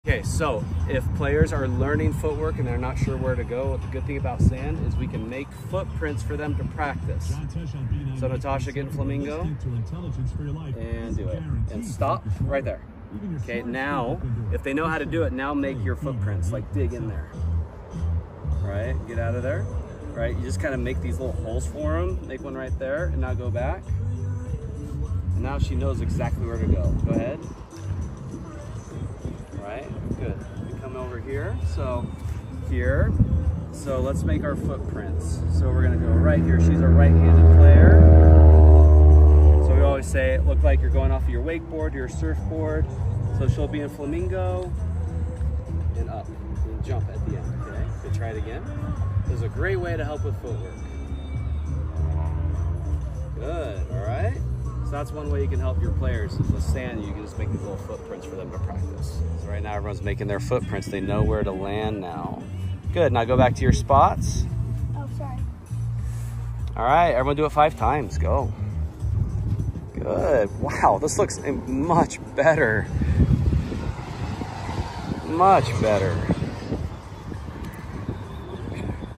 Attacking footwork for beginners - use foot prints !
Fun way to teach beginners on the beach